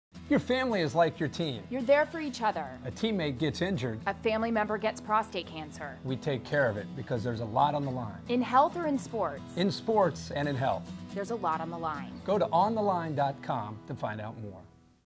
Erin Andrews and Kirk Herbstreit On The Line
Erin Andrews and Kirk Herbstreit tell us what's On The Line!  a commercial from the On The Line - Prostate Cancer Awareness Campaign.